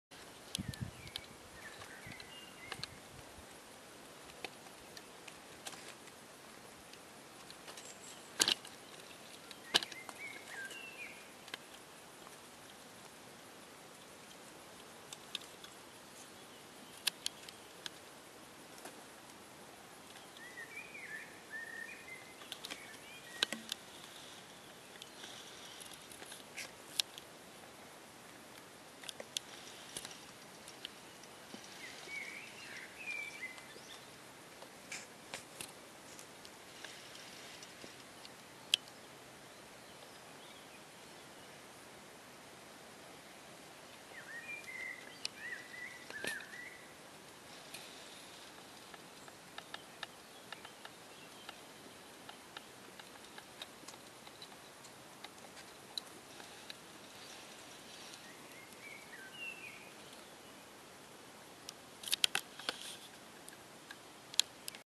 Limodorum abortivum(L.) Swartz-Pieria (Ano Milia)
Λιμόδωρο το εκτρωτικό (Limodorum abortivum). Μεγάλο φυτό χωρίς πράσινα φύλλα με μοβ άνθη.